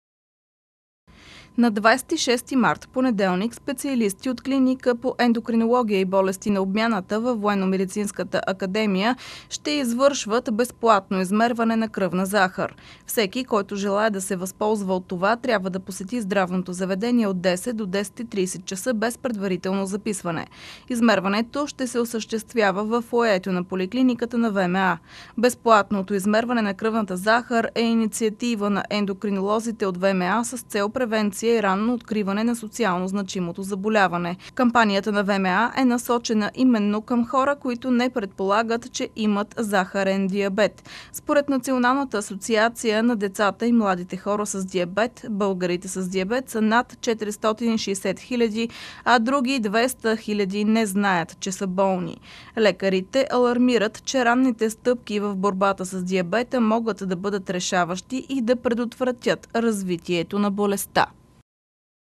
26 март. София. Безплатно измерване на кръвната захар във ВМА /25.03.2018 г./
На 26 март, понеделник, специалисти от Клиника по ендокринология и болести на обмяната във Военномедицинска академия ще извършват безплатно измерване на кръвна захар. Всеки, който желае да се възползва от това, трябва да посети здравното заведение от 10 до 10.30 часа без предварително записване. Измерването ще се осъществява във фоайето на поликлиниката на ВМА